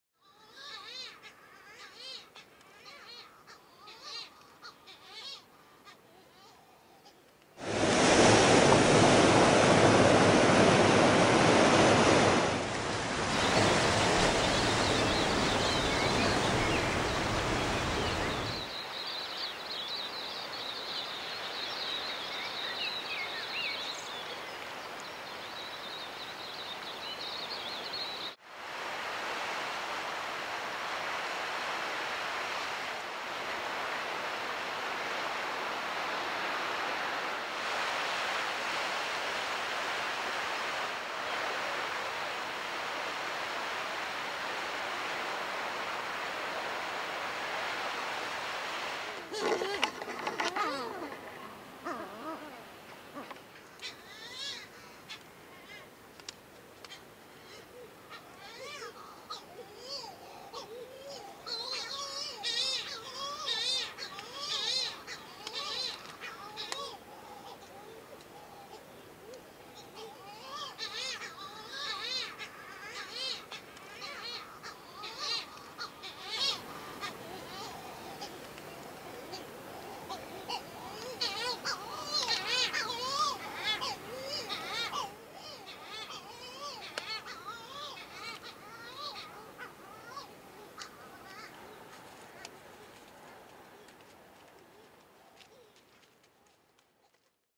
Titi at Te Rere
Titi have returned to the Forest and Bird Society's Te Rere penguin reserve in Southland. An outcome of the intensive predator control and forest restoration work that is being done there by the Southland Branch of the Society.